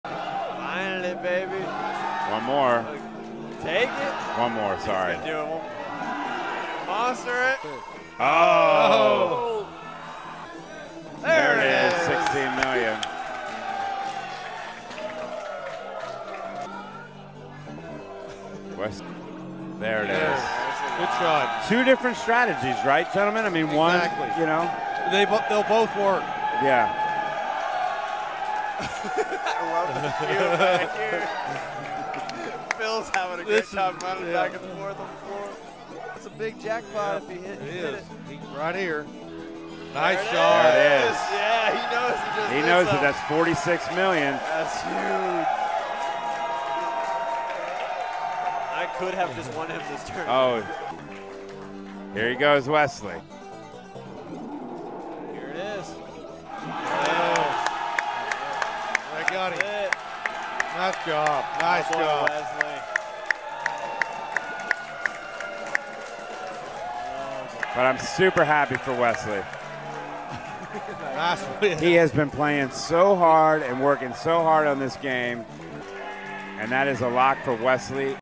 Houston does pinball tournaments differently!!
Get ready for the party energy for the Space City Open finals this weekend at the Houston Arcade Expo! Last year Wesley Johnson claimed the top spot. Who will be the Space City Champ this year? Tune into Twitch on Saturday evening to find out.